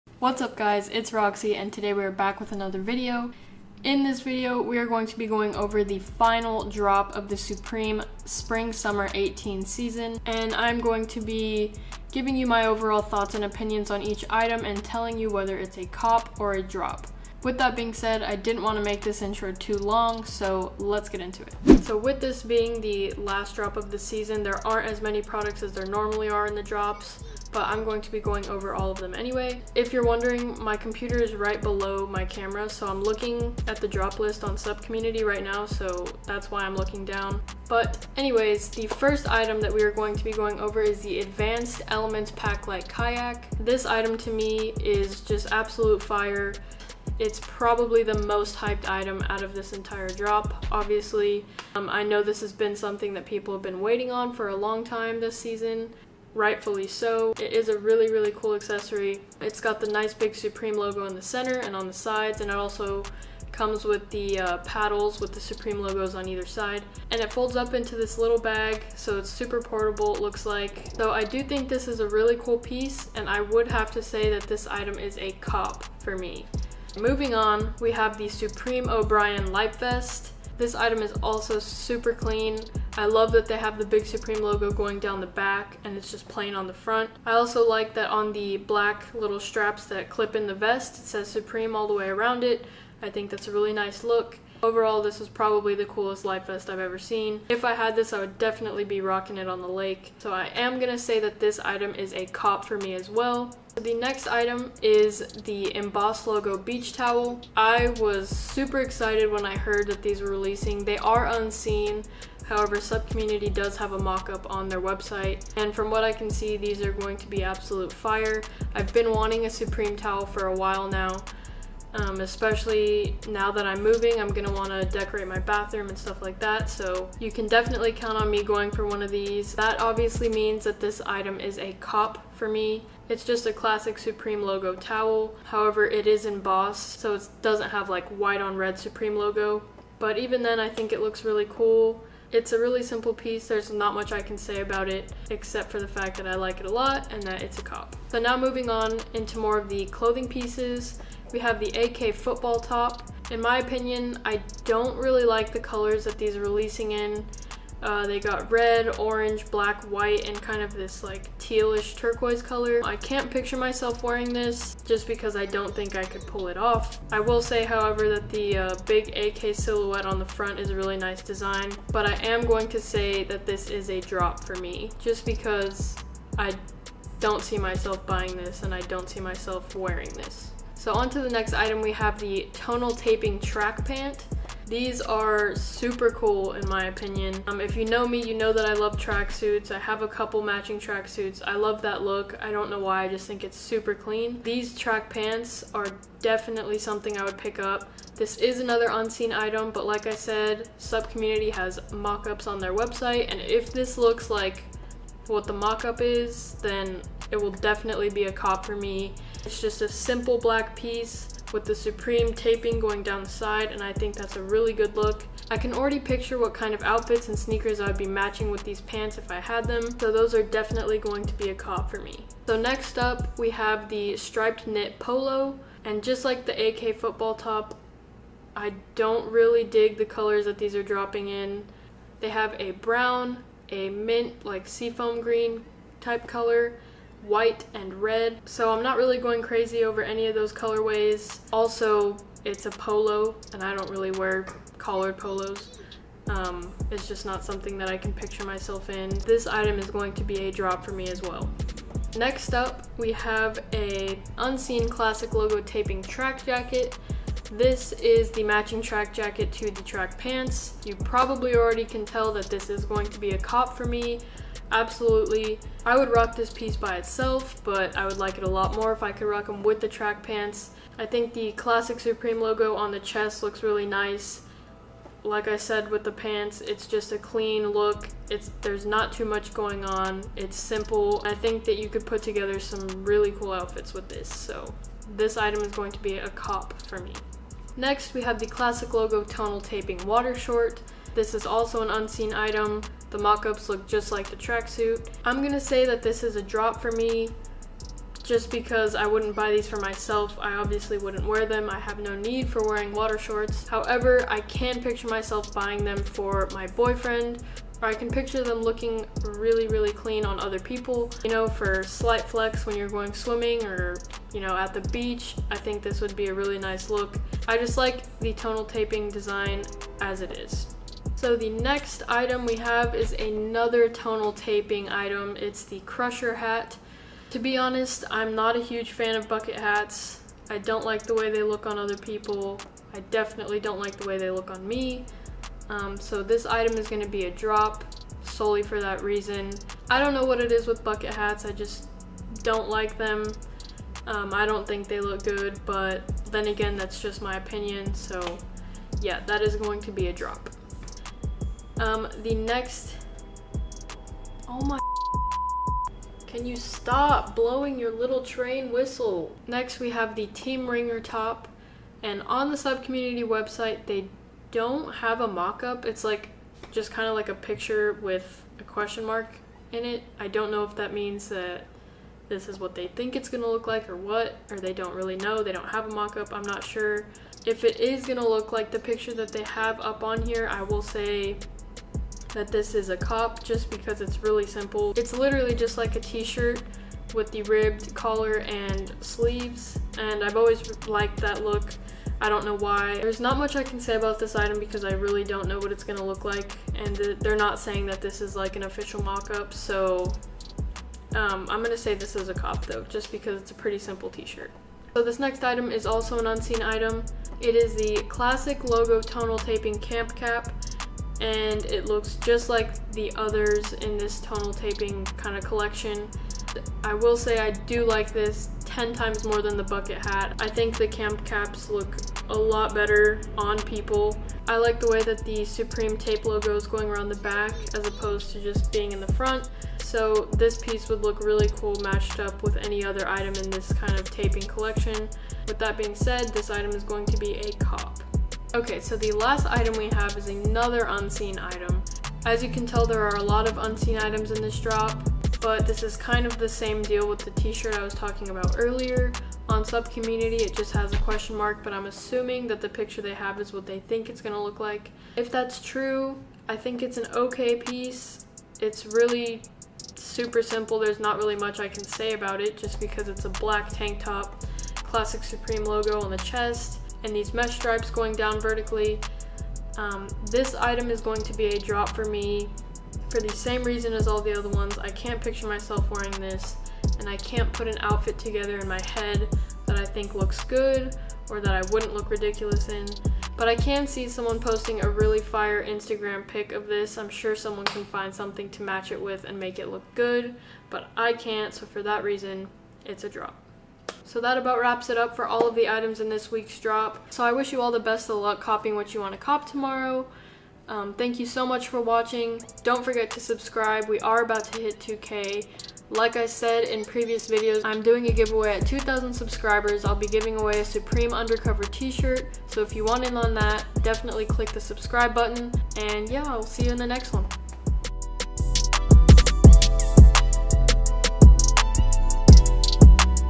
SUPREME SS'18 WEEK 20 COP OR DROP!
Hey everyone!! In this video, I go over the FINAL drop list for the supreme spring summer 2018 season, and tell you guys my opinion on all of the items coming out. I hope everyone can cook tomorrow on the things they want ;)
Anyway, thank you guys so much for watching. I APPRECIATE YOU!!!

Personal Instagram: @roxychaheine
Hype/Fashion Blog Instagram: @keepaustinhype